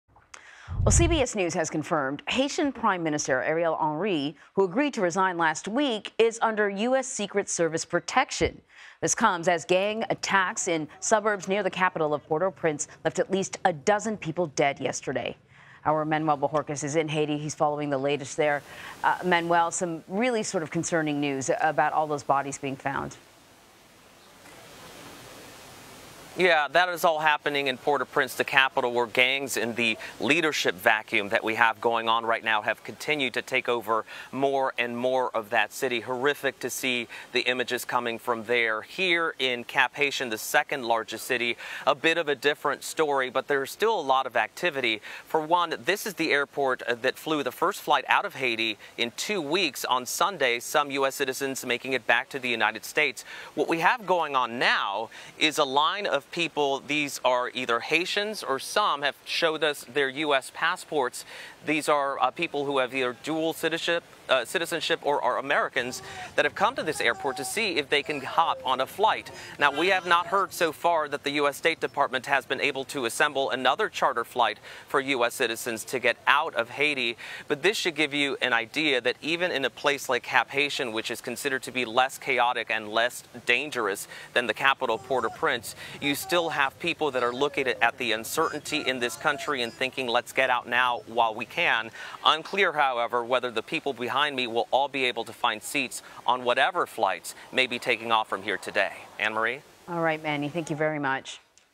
Haiti's prime minister under U.S. protection, at least 10 killed in Port-au-Prince suburb
Americans trapped in Haiti are desperately trying to escape the island nation as the conditions in Port-au-Prince continue to spiral amid gang violence. Meanwhile, CBS News has confirmed Haitian Prime Minister Ariel Henry, who agreed to resign last week, is under U.S. Secret Service protection. CBS News national correspondent Manuel Bojorquez has more.